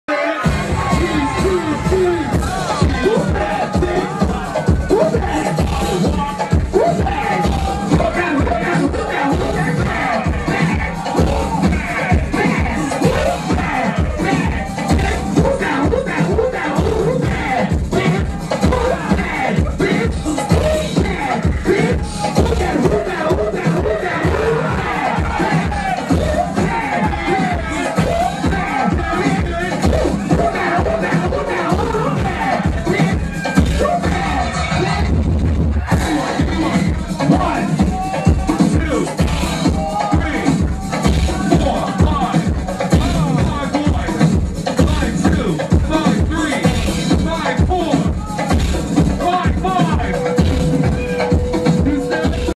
Kevin JZ Prodigy Vs Berlin Ebony
Team Philly Vs Team Chicago

State to State Team Performance

Liberty Awards Ball 2009 NYC